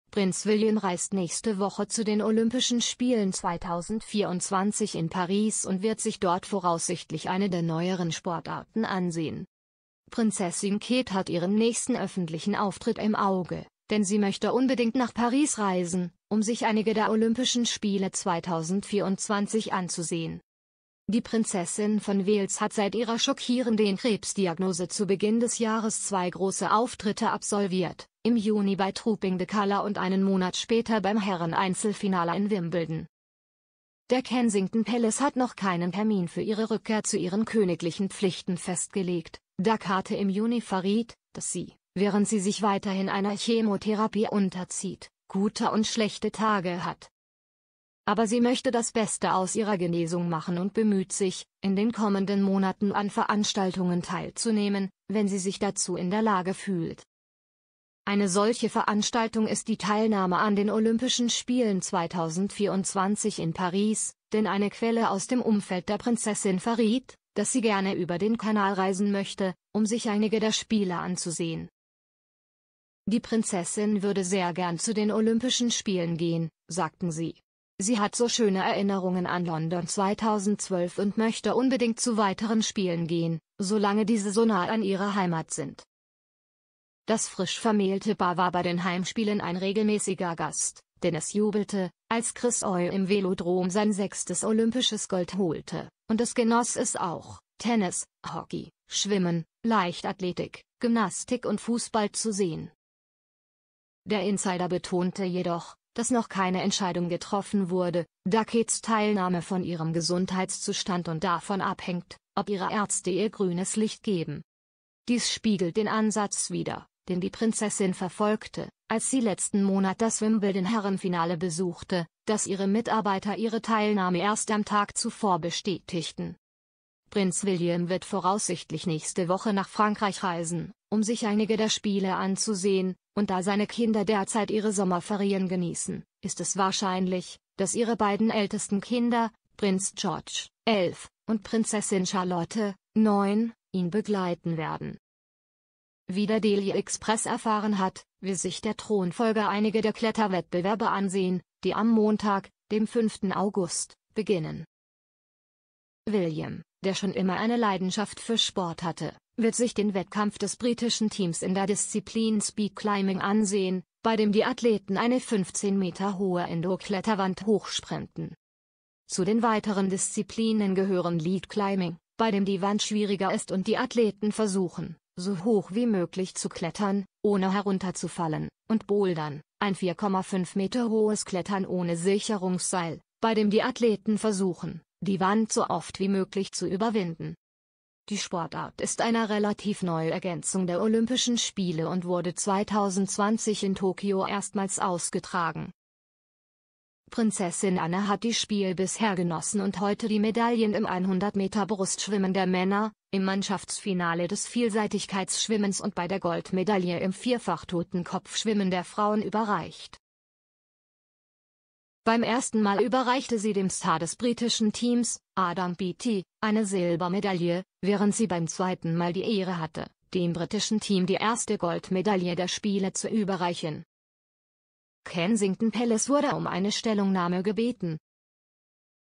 Prinzessin Kate plant in den nächsten Tagen einen großen öffentlichen Auftritt, während sie ihre Kr
Prinz William reist nächste Woche zu den Olympischen Spielen 2024 in Paris und wird sich dort voraussichtlich eine der neueren Sportarten im Programm ansehen.